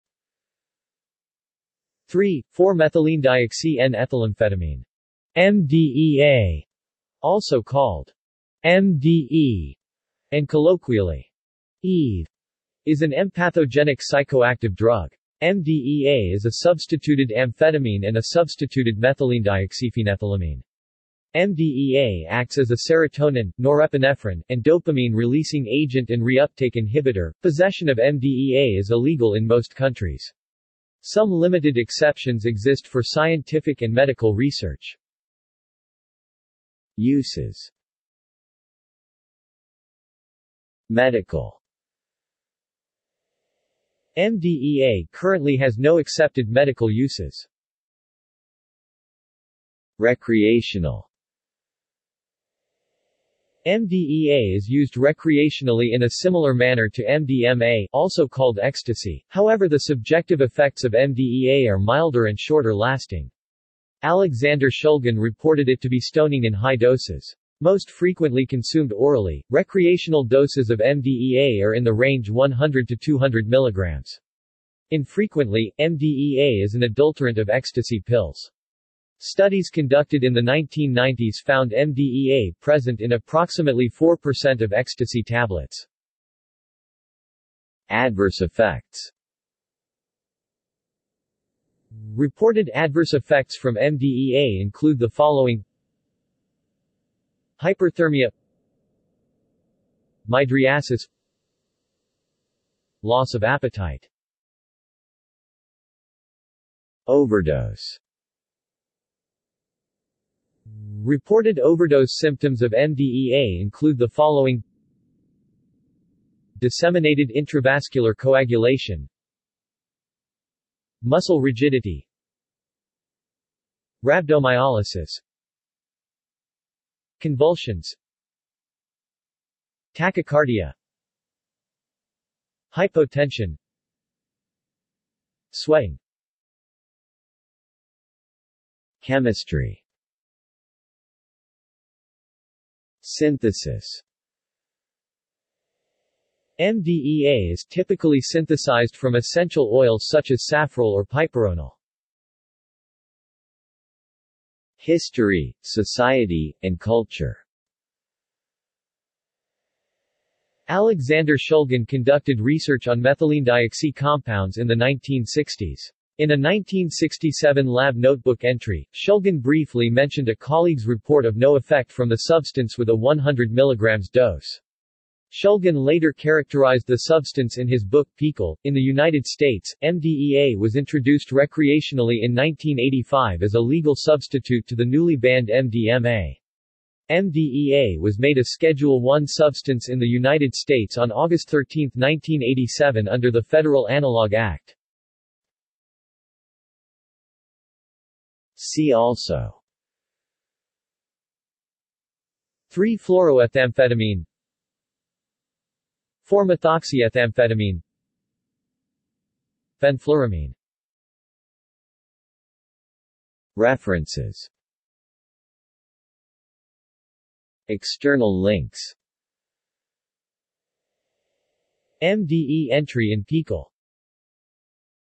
Methylenedioxyethylamphetamine | Wikipedia audio article
This is an audio version of the Wikipedia Article:
Methylenedioxyethylamphetamine

SUMMARY
3,4-Methylenedioxy-N-ethylamphetamine ("MDEA"; also called "MDE" and colloquially, "Eve") is an empathogenic psychoactive drug. MDEA is a substituted amphetamine and a substituted methylenedioxyphenethylamine. MDEA acts as a serotonin, norepinephrine, and dopamine releasing agent and reuptake inhibitor.Possession of MDEA is illegal in most countries. Some limited exceptions exist for scientific and medical research.